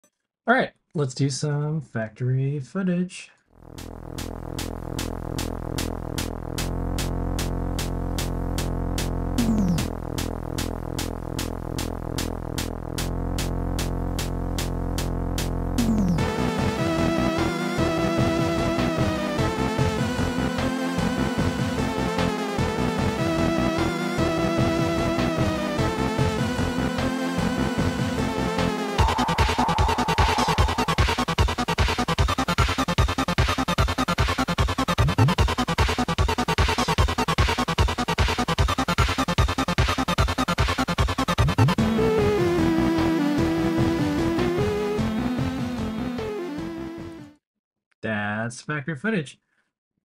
Advanced Manufacturing: Made in NYC by Adafruit 9/20/2023
We’re back this week with more behind the scenes manufacturing footage!